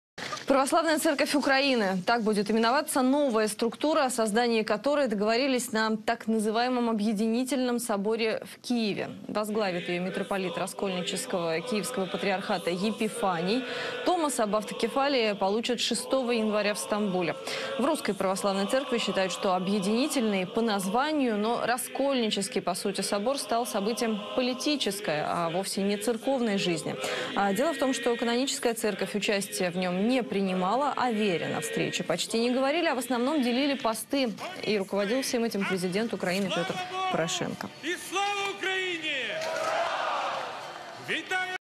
Новую церковь на Украине назвали Православной
Православная церковь Украины - так будет именоваться новая структура, о создании которой договорились на так называемом "объединительном соборе" в Киеве. Возглавит ее митрополит раскольнического Киевского патриархата Епифаний. Томос об автокефалии получат 6 января в Стамбуле.